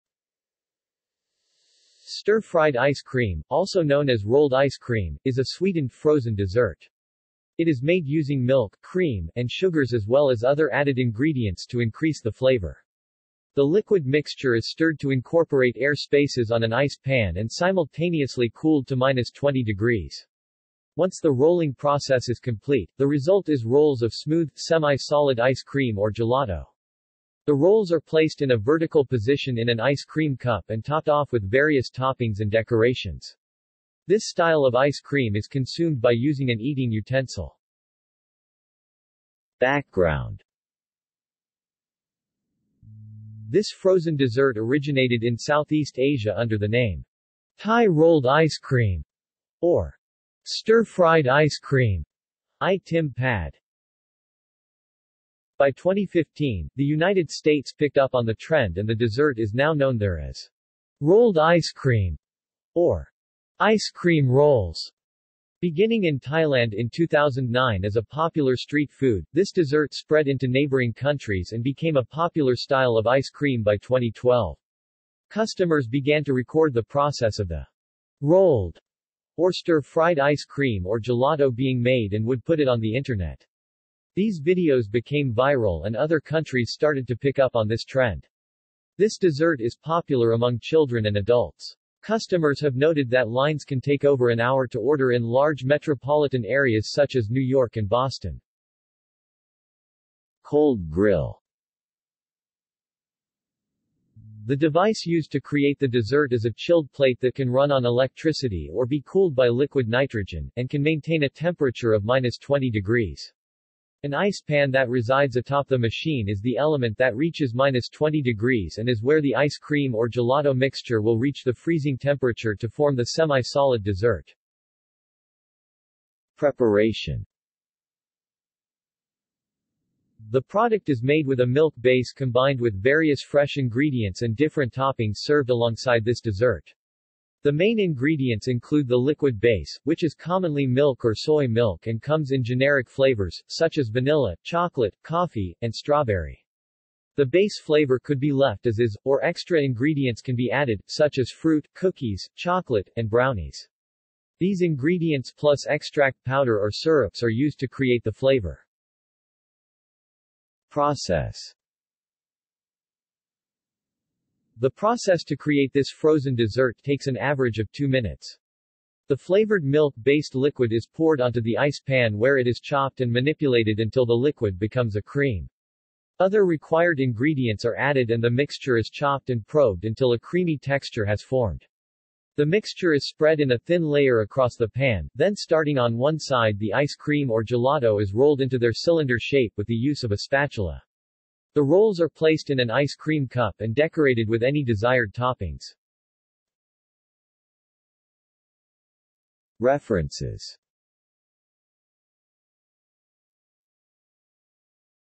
Stir-fried ice cream | Wikipedia audio article
This is an audio version of the Wikipedia Article:
Stir-fried ice cream

SUMMARY
Stir-fried ice cream, also known as rolled ice cream, is a sweetened frozen dessert. It is made using milk, cream, and sugars as well as other added ingredients to increase the flavour. The liquid mixture is stirred to incorporate air spaces on an ice pan and simultaneously cooled to -20 degrees. Once the rolling process is complete, the result is rolls of smooth, semi-solid ice cream or gelato. The rolls are placed in a vertical position in an ice cream cup and topped off with various toppings and decorations. This style of ice cream is consumed by using an eating utensil.